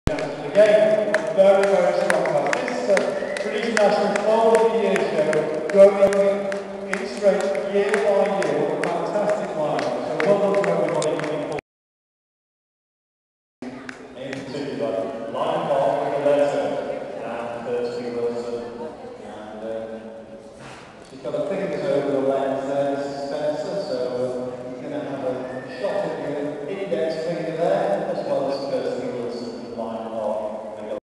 Bathley Hill Farm Stud - Daisy British National Foal Of The Year Show 2009
Daisy Flint by Simply The Best (G2ID) 2nd Competition Horse